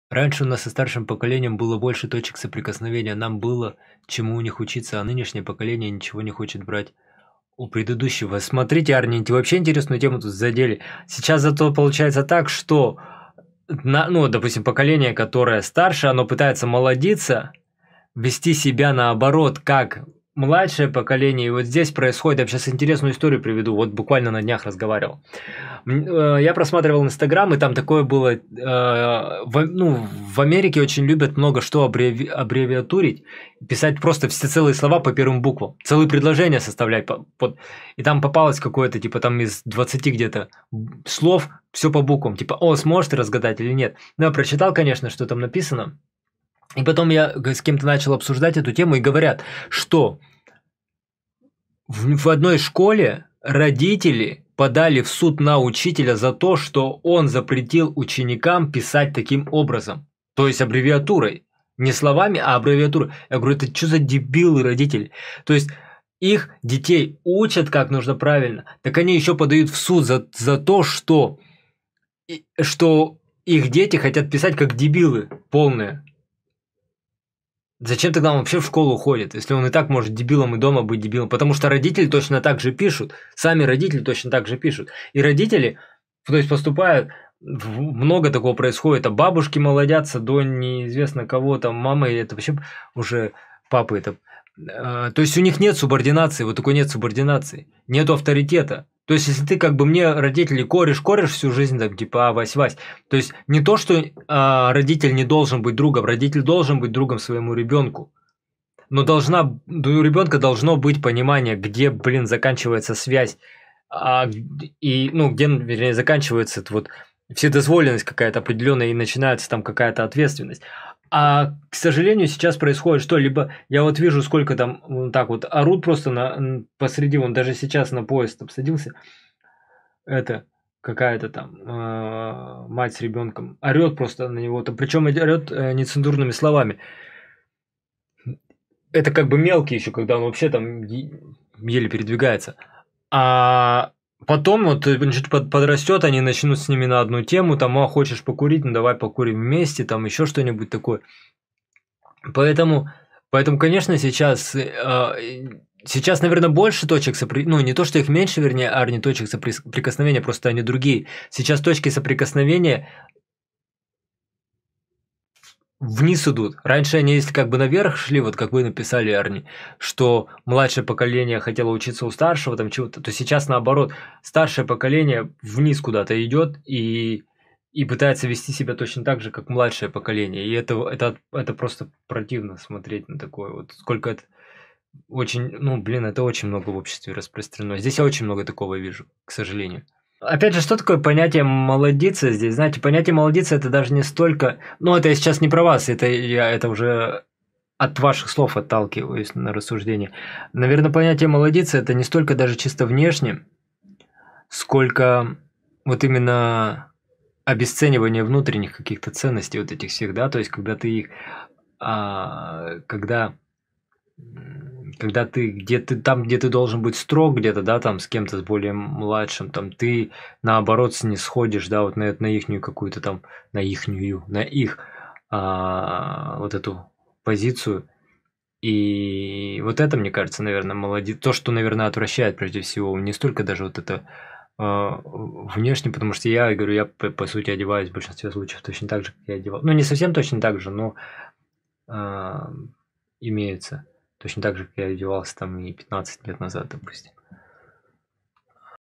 "Молодиться" - это обесцениваться внутренне, а не просто внешне измениться.
Arny3452 затронул серъёзный момент касаемо отстутствия точек соприкосновений между старшим и младшим поколением.
Что вы думаете на этот счёт?